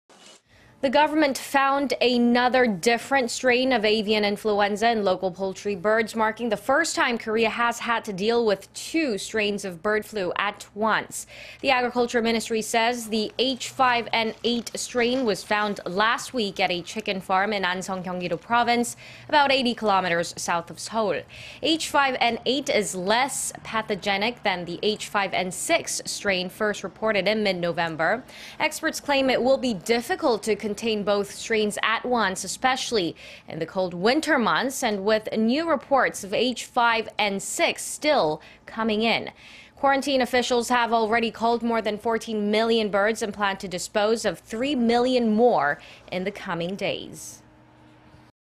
Korea confirms second strain of bird flu at poultry farm
두가지 형태 AI 국내 첫 동시 발생, 방역에 악재
The government found another, different strain of Avian Influenza in local poultry birds, marking the first time Korea has had to deal with two strains of bird flu at once.
The agriculture ministry says the H-5-N-8 strain was found last week at a chicken farm in Anseong, Gyeonggi-do Province... about 80 kilometers south of Seoul.
H-5-N-8 is less pathogenic than the H-5-N-6 strain first reported in mid-November.
Experts claim it'll be difficult to contain both strains at once, especially in the cold winter months, and with new reports of H-5-N-6 still coming in.
Quarantine officials have already culled more than 14 million birds and plan to dispose of three million more in the coming days.